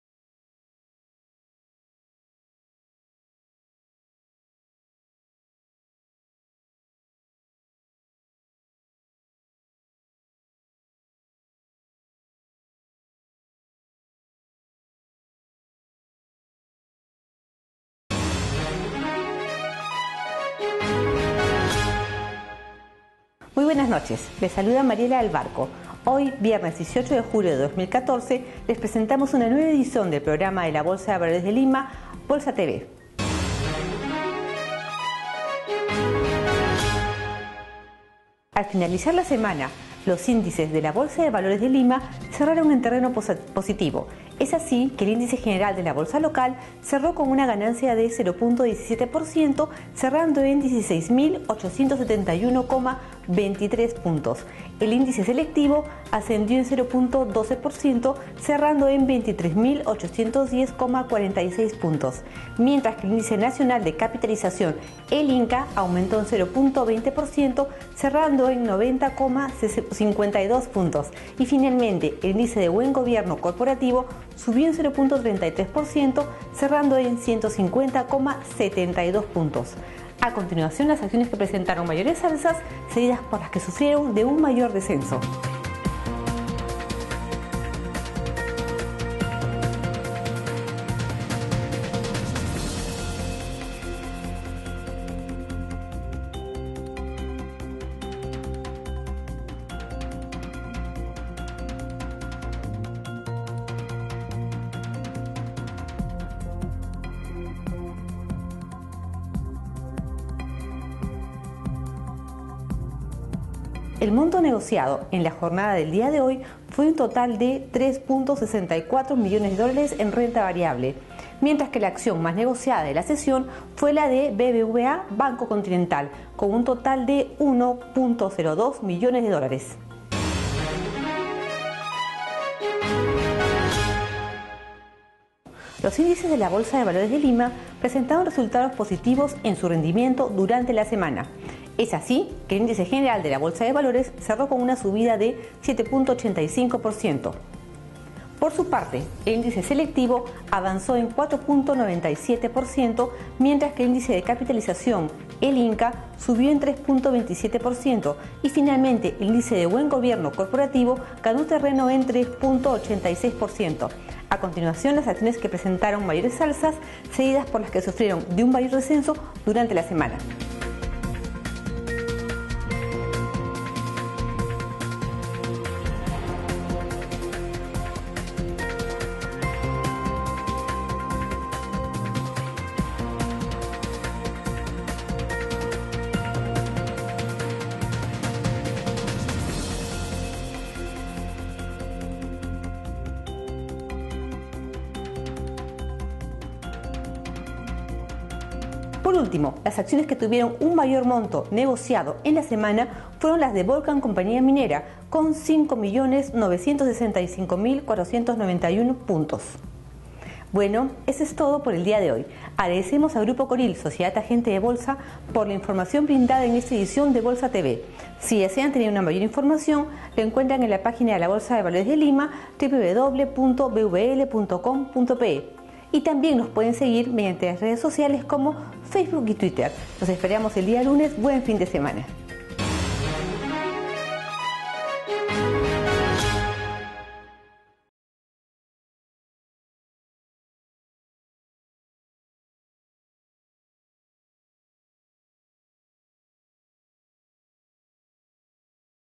BOLSA TV (18/07/2014)
Segmento en línea sobre la Bolsa de Valores de Lima.
Conoce en tan sólo minutos las principales noticias de la jornada bursátil del día.